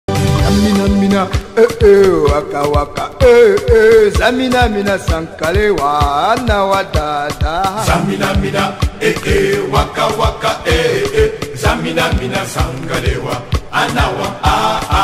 Stick or insect? ❤️ Stick insect
Stick insect are not aggressive or venomous. They are weak clumsy fliers, usually flying for short bursts. They are experts at camouflage. They are also capable of remaining motionless for long periods of time.
These fascinating creatures are simply peace-loving vegetarians who like to keep to themselves@gistzillion